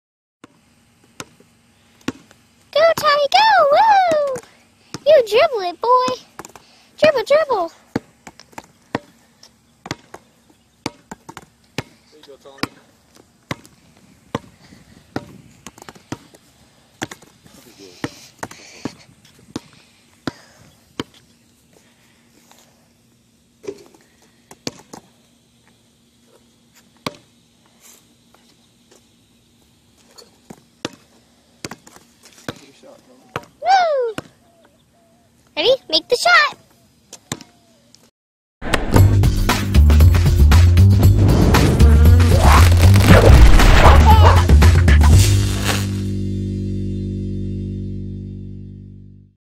Capuchin monkey loves to play basketball
Tommy the capuchin monkey has made it clear what his favorite sport is as he dribbles a basketball around the pavement. Even though the ball is almost as big as him, Tommy won't let that stop him from practicing his skills!